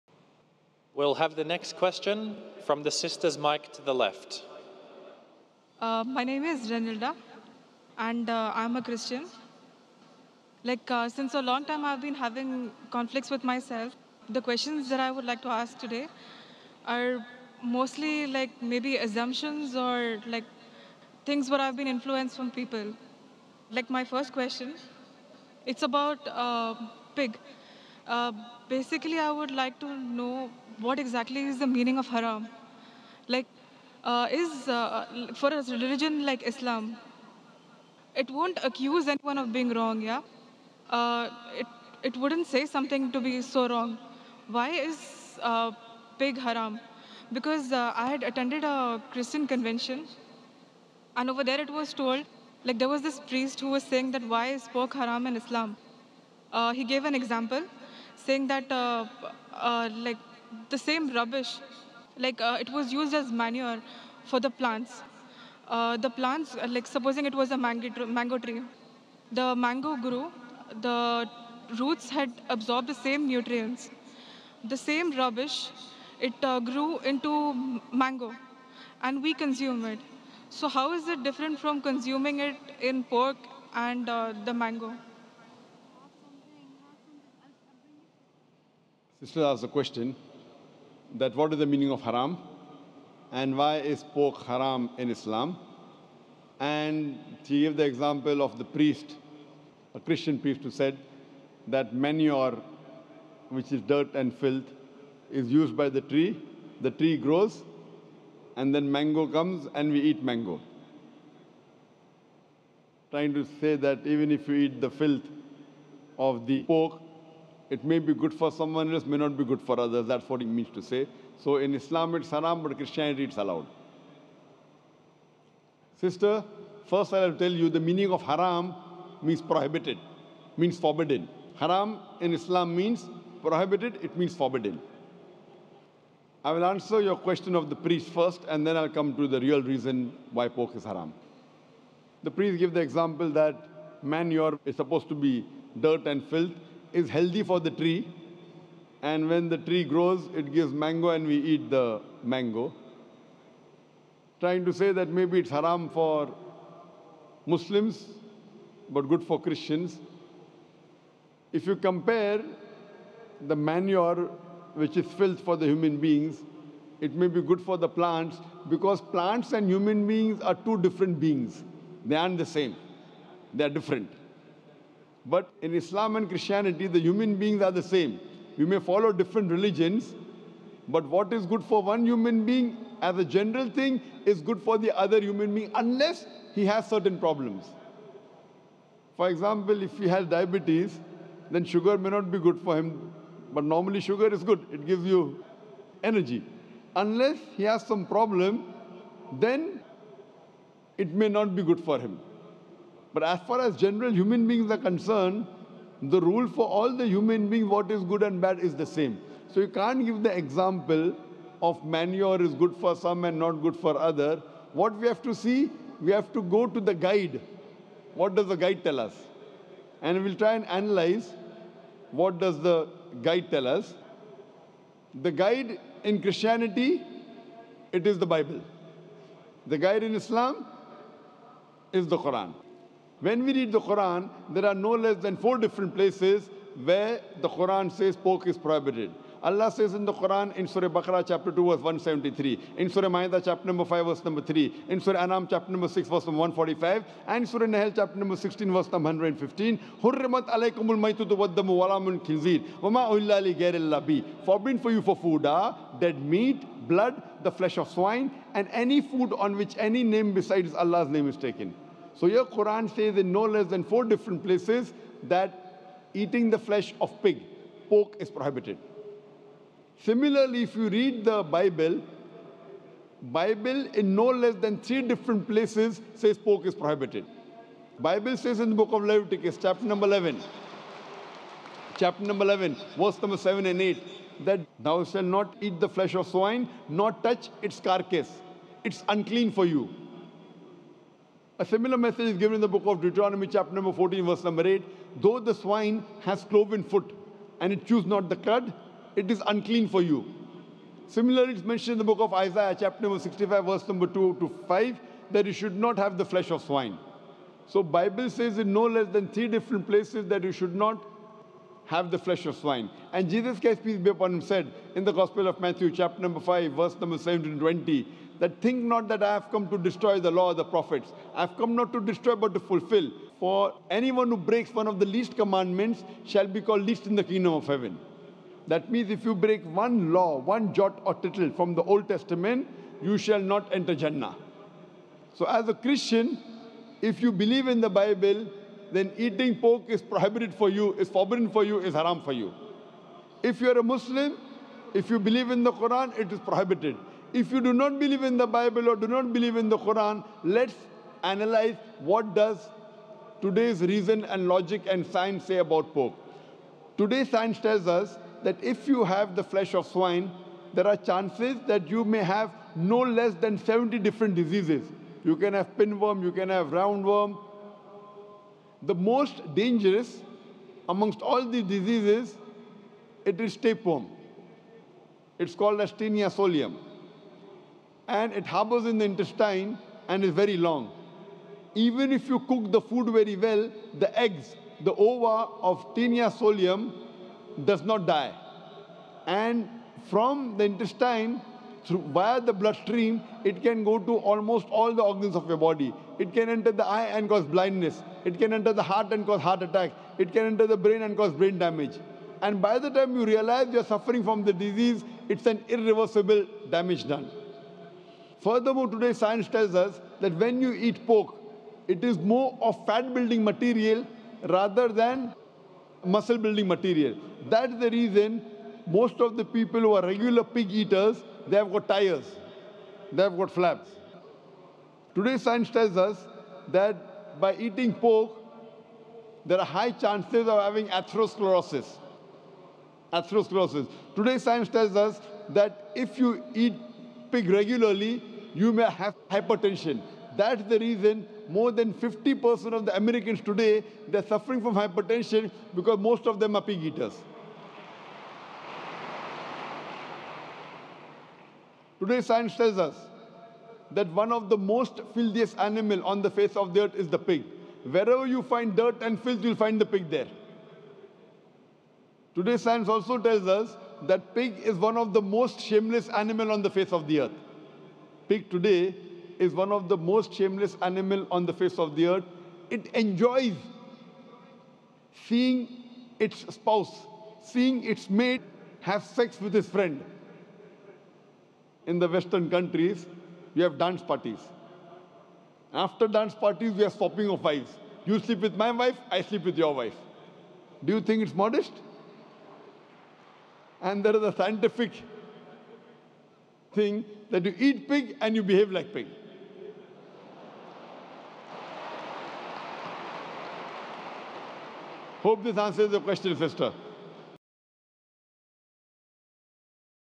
Kenapa Babi Haram di Islam ?! Dr. Zakir Naik
Akan ada meme kata - kata bijak dan motivasi dari Dr. Zakir Naik dalam halaman Facebook kami, silahkan Like

Dalam lanjutan video tanya jawab Dr. Zakir Naik di Dubai. Seorang wanita Kristen berkata bahwa Babi itu tidak haram, babi bisa dimakan oleh semua orang, babi bisa dimakan juga oleh orang Islam.

Credit to : @Drzakirchannel